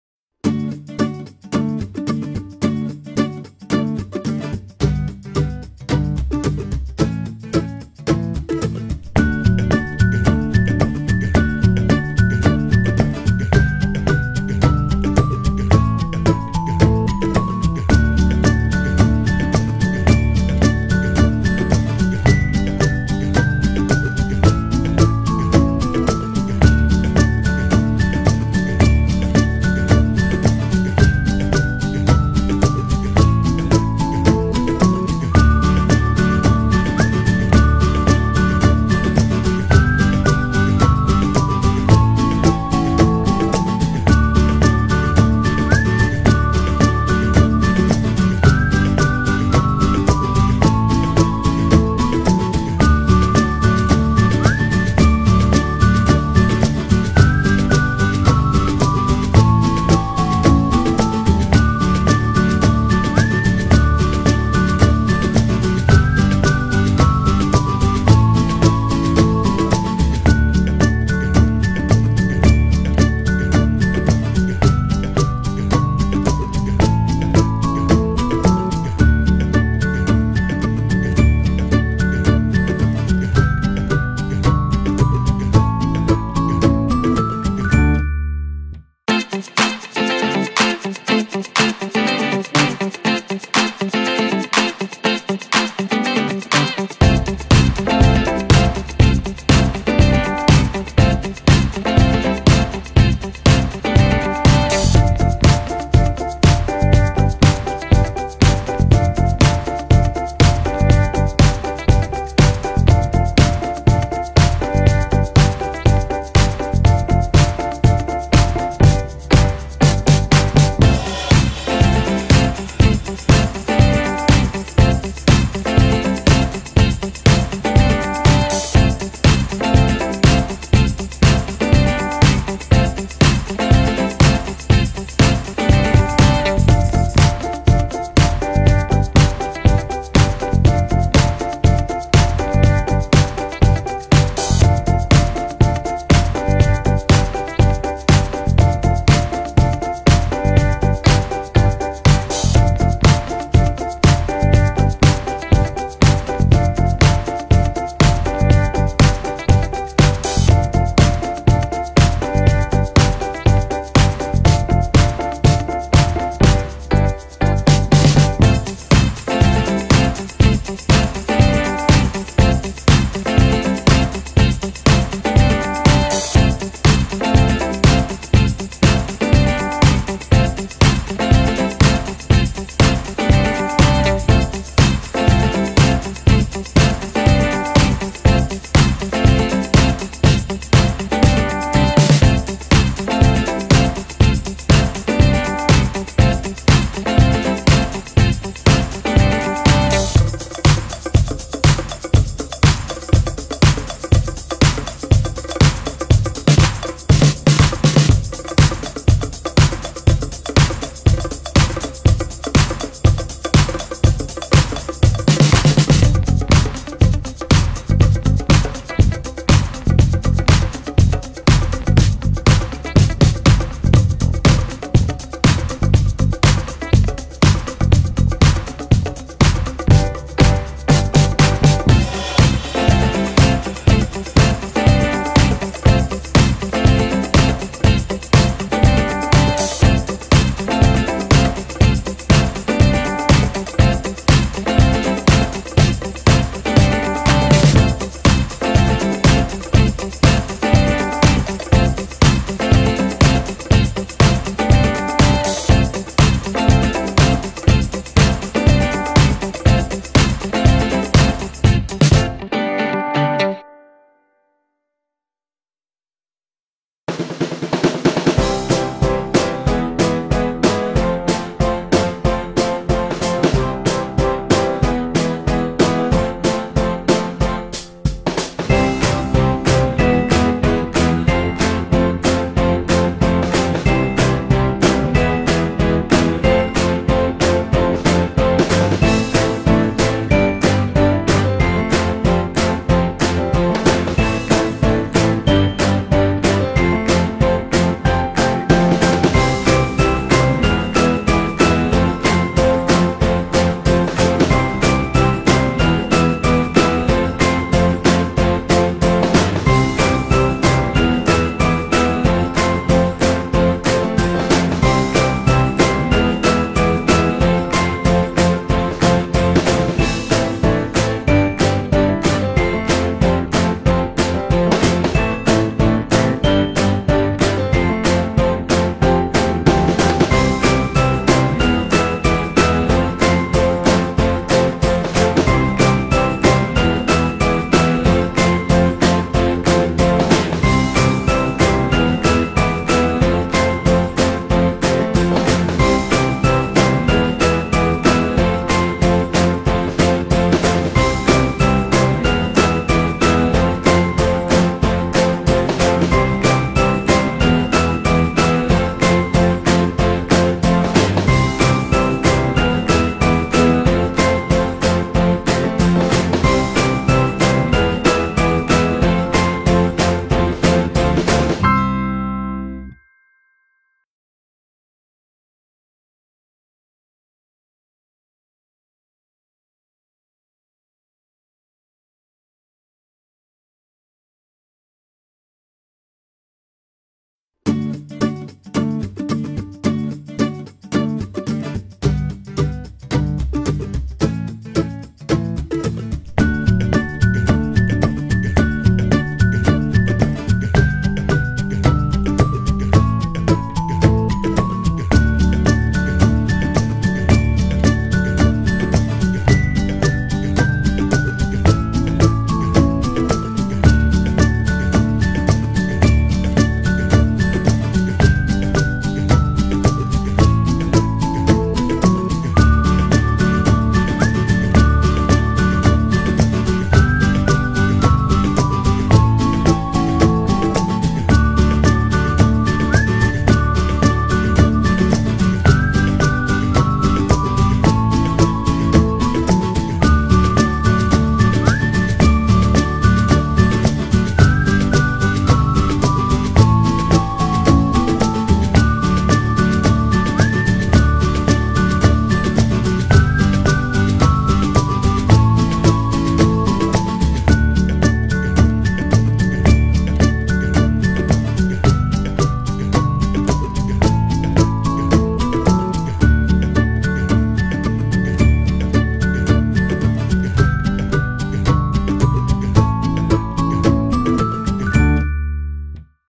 MDM4U ISU Survival Guide
This video is a "survival guide" for ISU writing for the Data Management course in my classroom, located in Mississauga, Ontario.

There is no verbal narrative for this video, it is just a screenshot of a Power Point presentation with public domain music (Thanks, YouTube!) overlaid on top. Screenshot with Screencast-O-Matic; music track added in Adobe Premiere Elements, with some minor trimming.

The video is slightly choppy, but that might be due to the animation from PowerPoint done during a screenshot, which can slows things down.

My students appreciated it, so teachers are free to use this video for their classrooms.